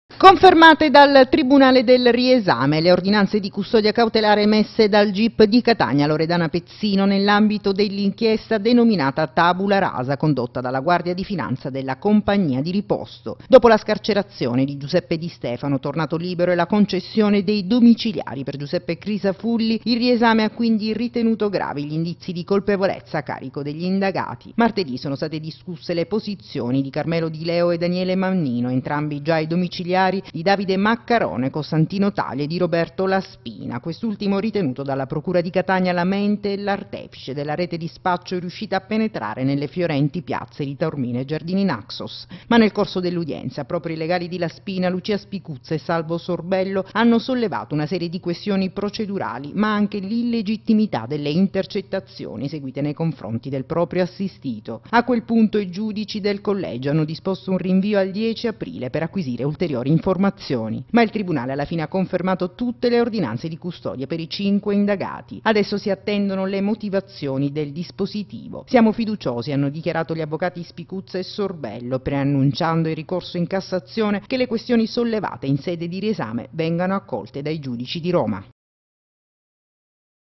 Cronaca giudiziaria. "Tabula rasa", provvedimenti confermati.
Confermate le ordinanze di custodia cautelare per gli altri cinque indagati nell'ambito dell'inchiesta denominata "Tabula rasa". Dopo un rinvio di due giorni per approfondimenti, i giudici del Riesame si sono pronunciati.